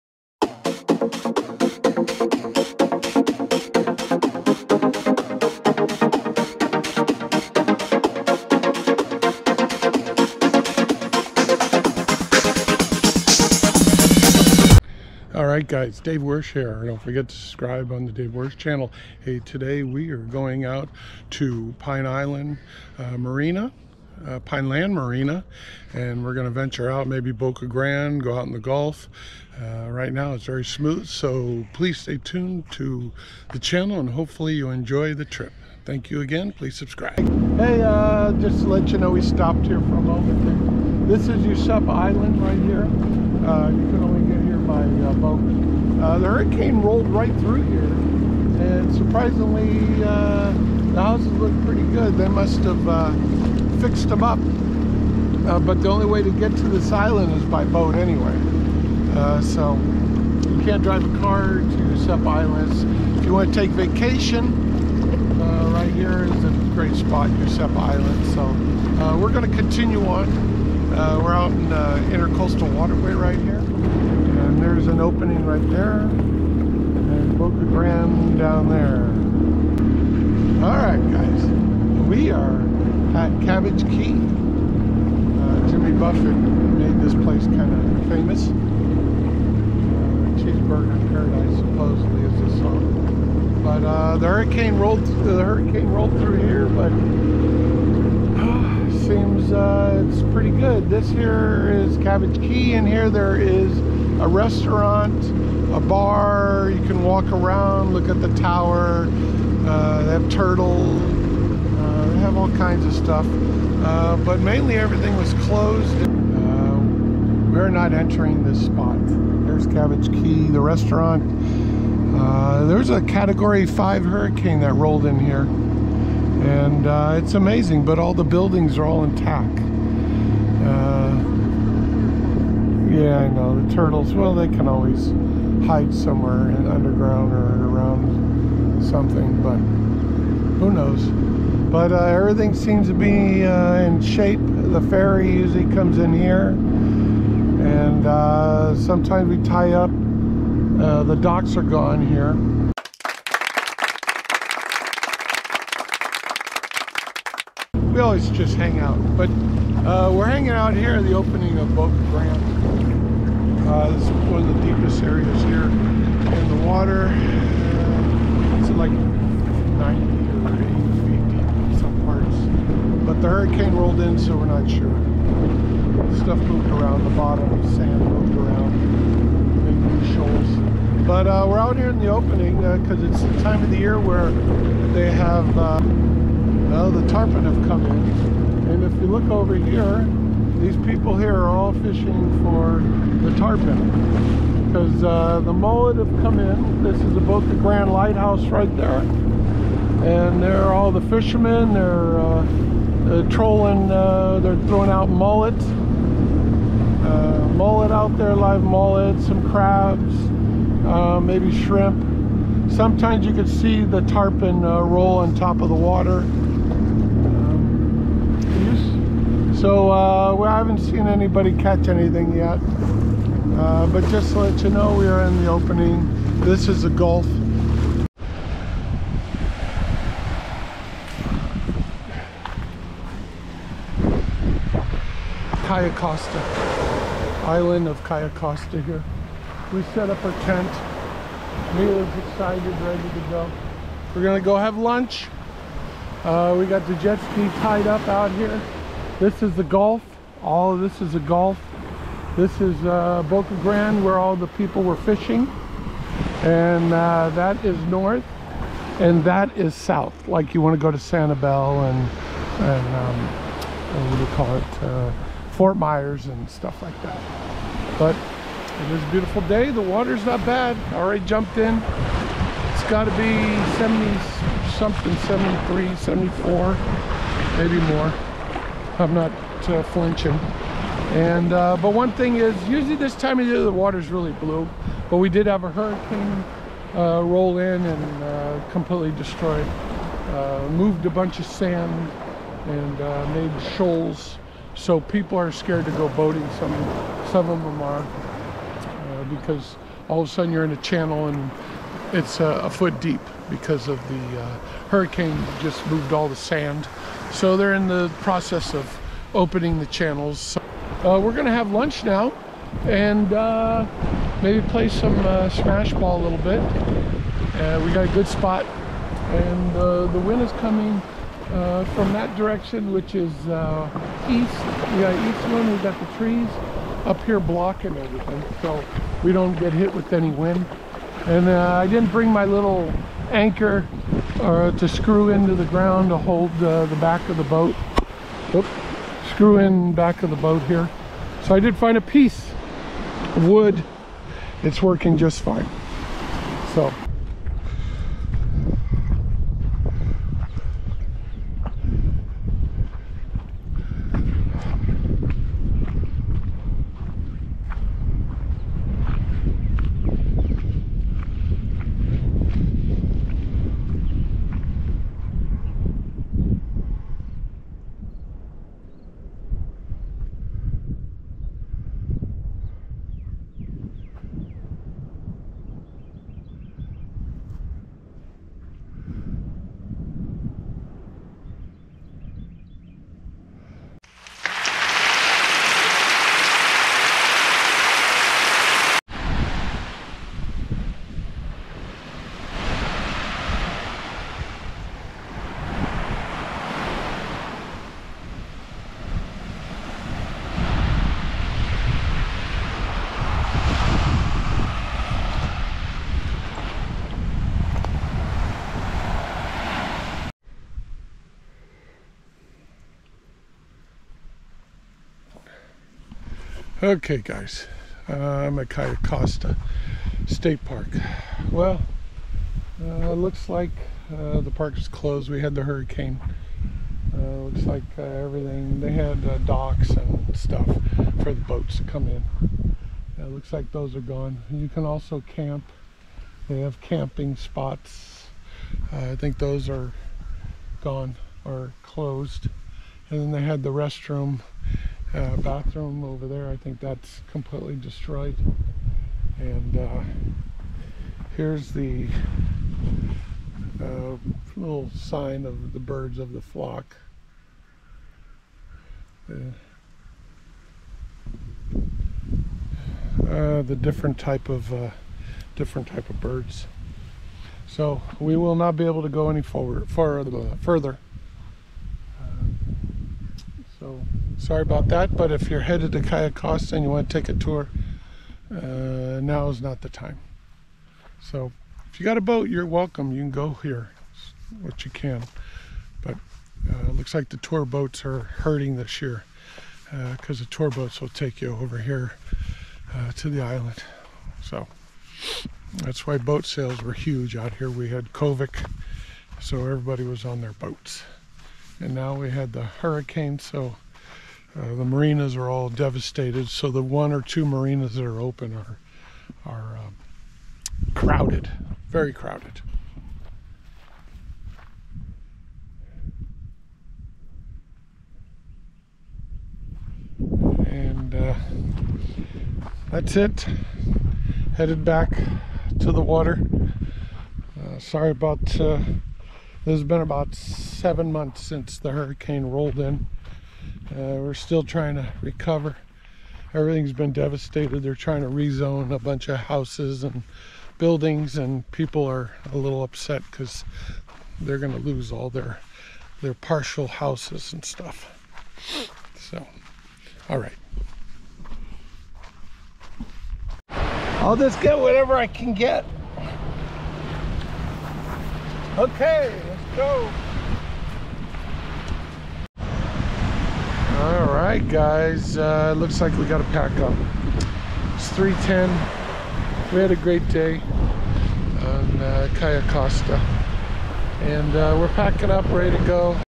Cayo Costa State Park still closed 6 months after hurricane and we jet ski to the beach
Spent day driving on jet ski to Cabbage Key,Useppa Island,Bocca Grand, and Cayo Costa beach touring around.